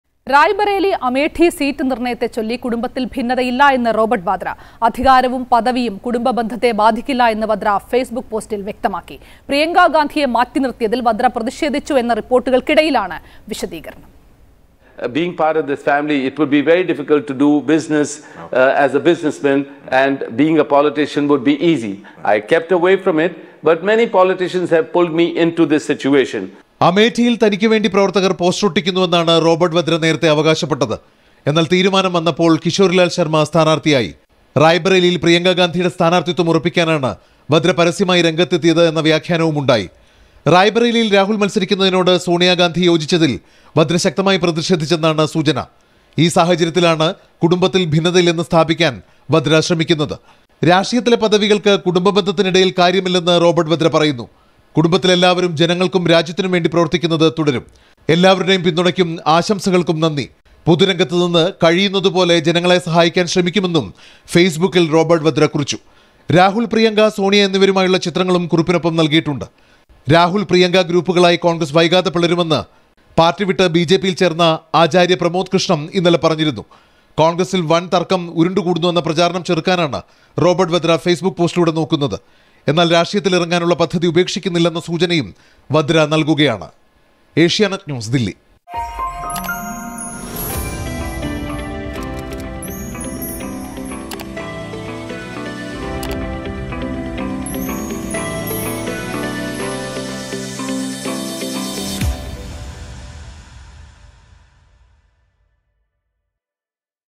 'അധികാരവും പദവിയും കുടുംബ ബന്ധത്തെ ബാധിക്കില്ല'; സീറ്റിനെച്ചൊല്ലി കുടുംബകലഹമില്ലെന്ന് വദ്ര
'അധികാരവും പദവിയും കുടുംബ ബന്ധത്തെ ബാധിക്കില്ല'; സീറ്റിനെച്ചൊല്ലി കുടുംബ കലഹമില്ലെന്ന് റോബർട്ട് വദ്ര